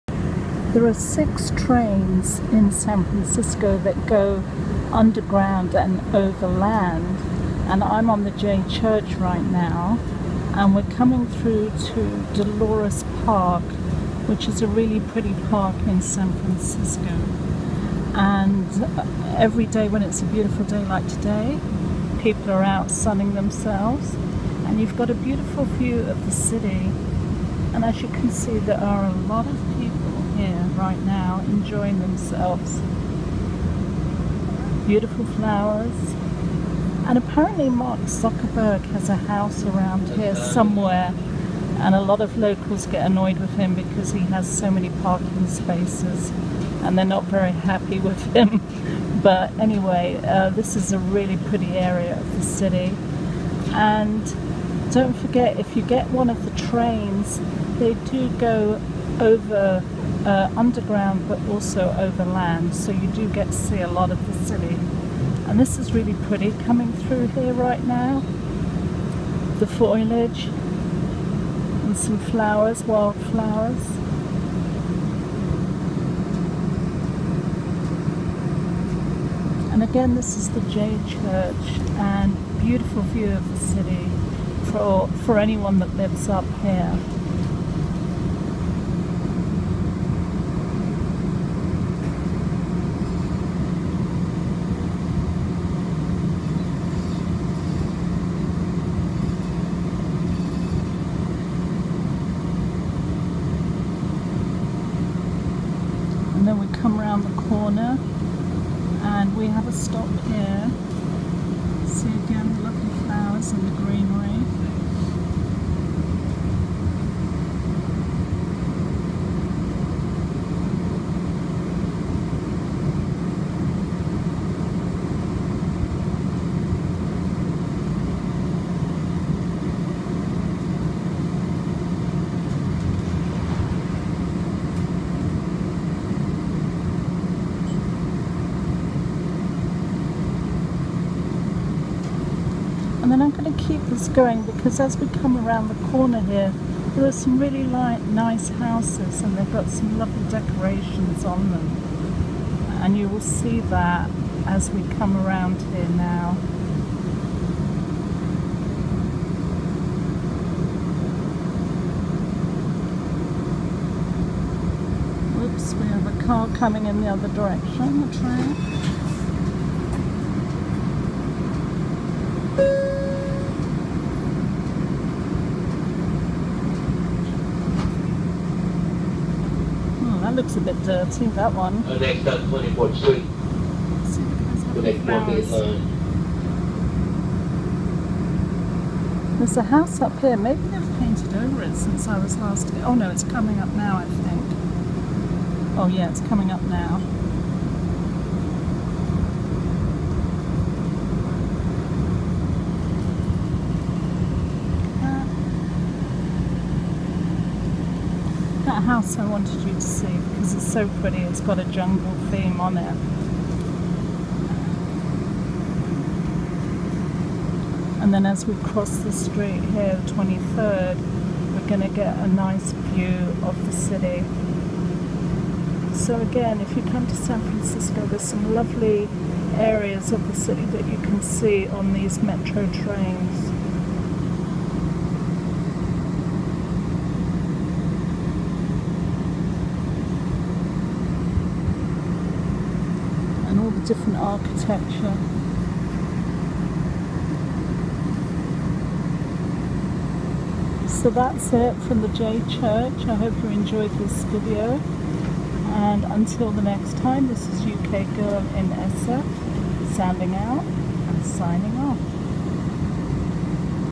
ukgirlinsf: Metro train journey in San Francisco
Dolores Park & a few street scenes on San Francisco's J Church metro train. Dirty, stained window so excuse the quality.